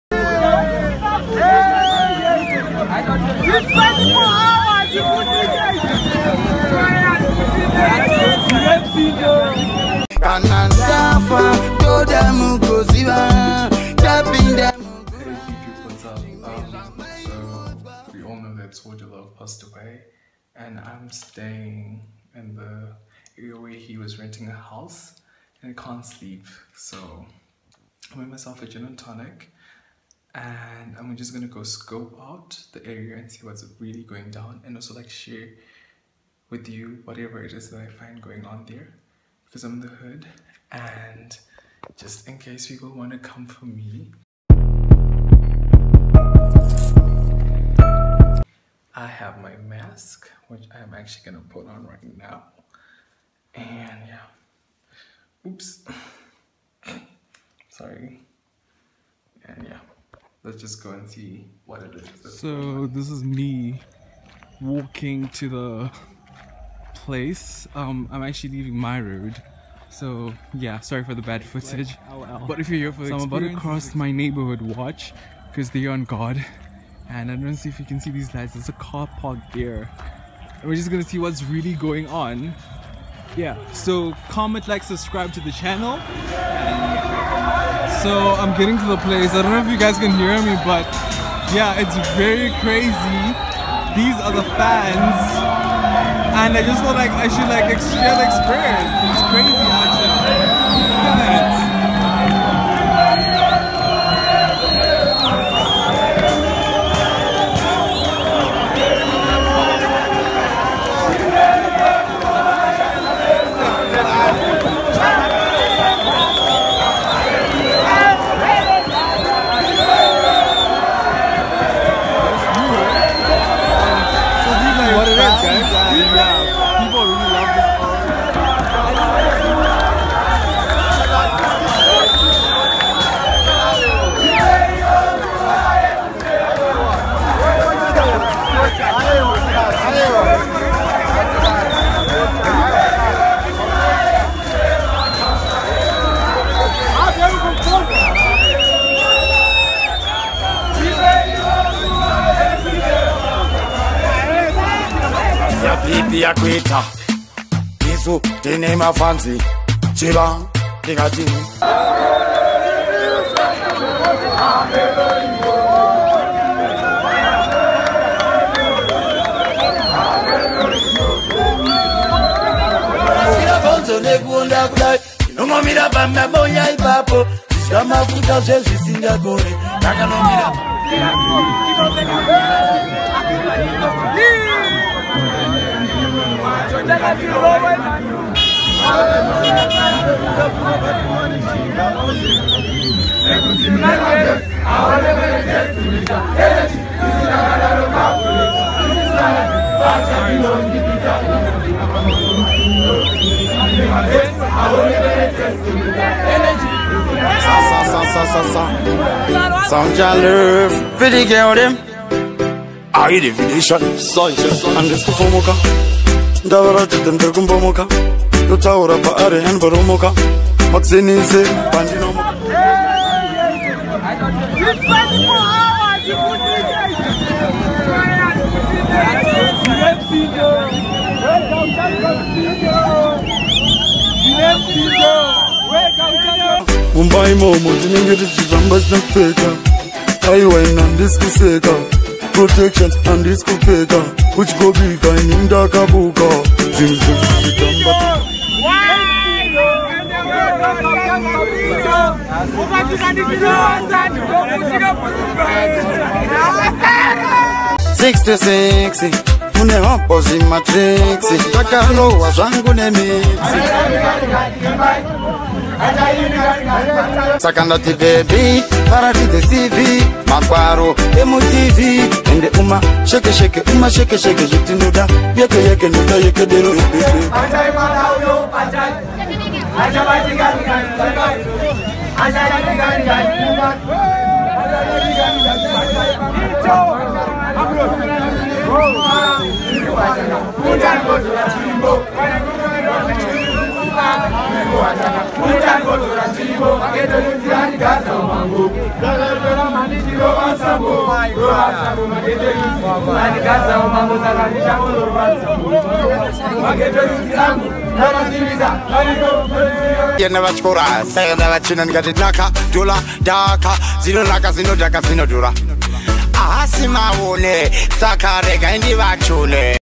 REMEMBERING SOUL JAH LOVE || ZIMBABWEAN YOUTUBER
REMEMBERING SOUL JAH LOVE

In this video I bring to you special send off to Soul Jah Love, I got to capture some of his fans who were mourning and celebrating his life the night before his burial, feel free to converse with me in the comment section and yeah SUBSCRIBE!!!

STAY SAFE -STAY HOME - SPREAD LOVE.

Any external imagery contained in this video is not mine, and I added it for creativity and humour sake.

All music video footage is courtesy of: Soul Jah Love.

-madok101.